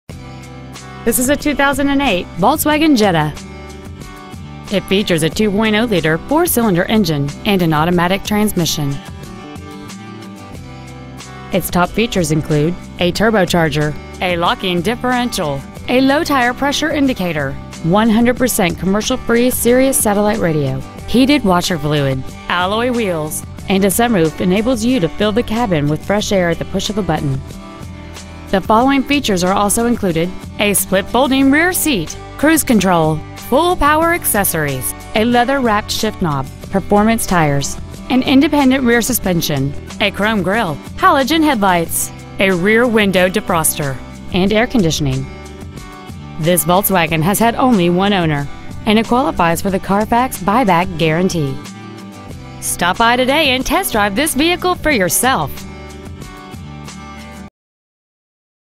Used 2008 Volkswagen Jetta Sedan Dallas TX 75207
We are proud to present this 2008 Volkswagen Jetta Sedan .

 Year : 2008
 Make : Volkswagen
 Model : Jetta Sedan
 Engine : 2.0L DOHC FSI turbocharged I4 engine
 Trans . : Automatic
 Exterior : Gray
 Miles : 71,640
 Interior : Gray